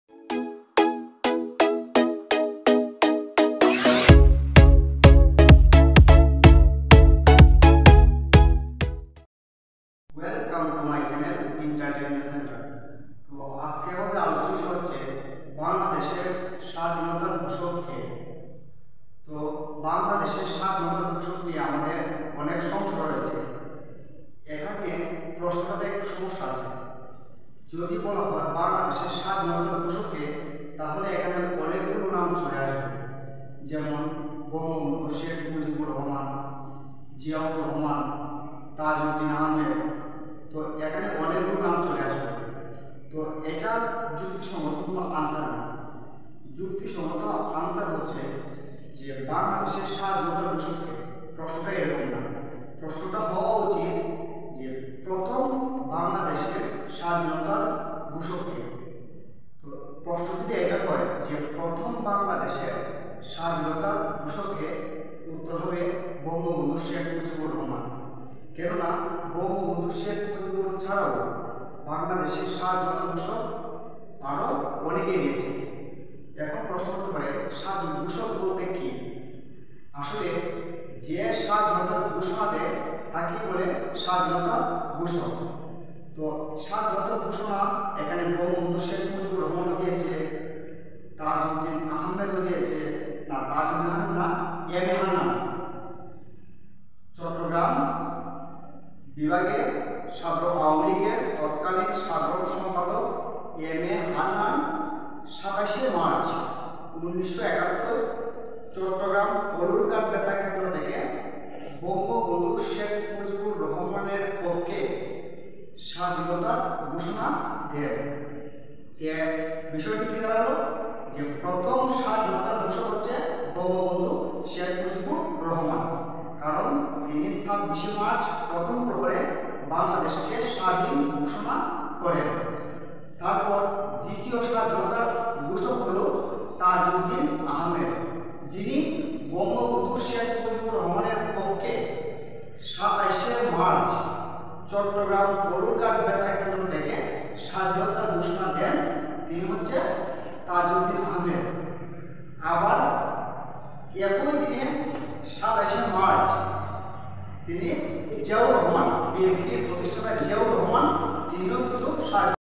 বাংলাদেশের স্বাধীনতার ঘোষক কে? Entertainment center.
In This video I am going to show, who is father of nation.